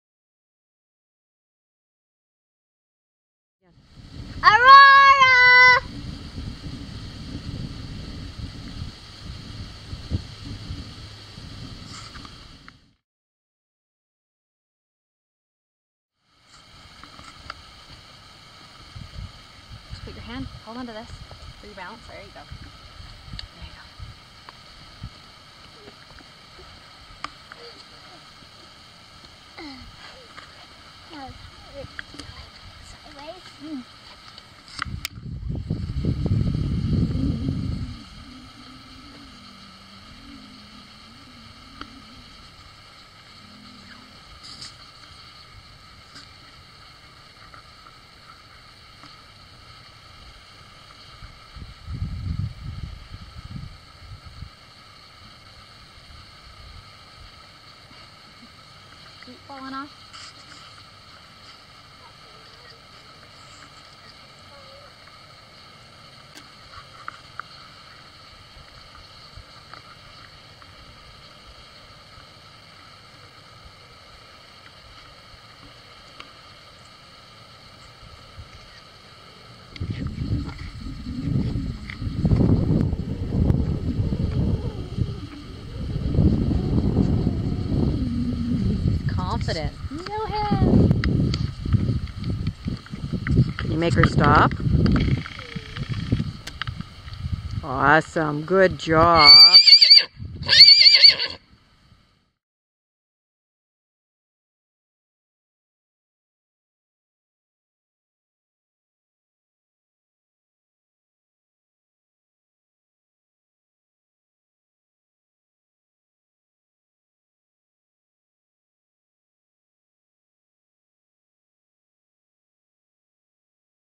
Aurora the Unicorn Three
This video is about Aurora the Unicorn with her horn - showing her with a 6 year old beginner under instruction.  For sale - $2,000 CDN